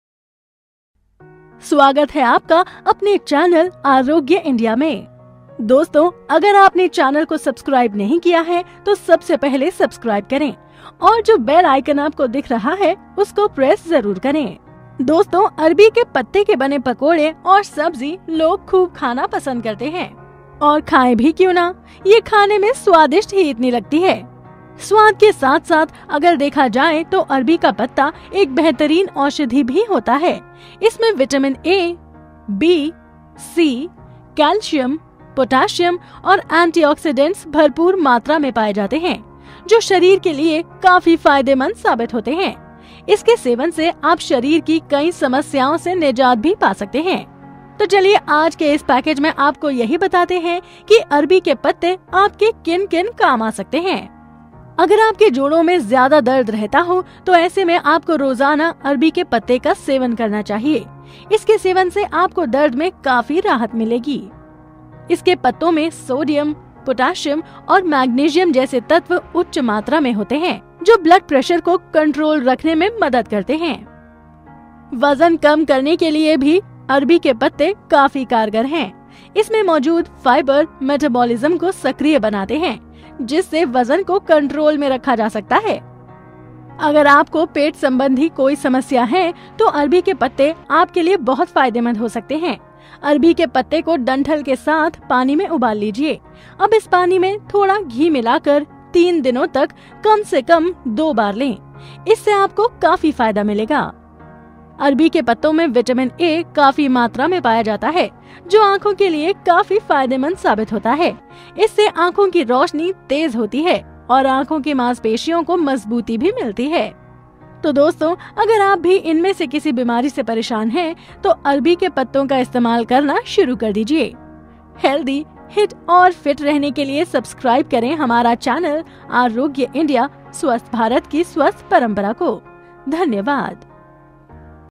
जोड़ो का दर्द,ब्लड प्रैशर,वजन कम,पेट से जुड़ी परेशानी,आंखों की रोशनी इन सबका एक रामबाण है ये पत्ते
Copyright : Arogya India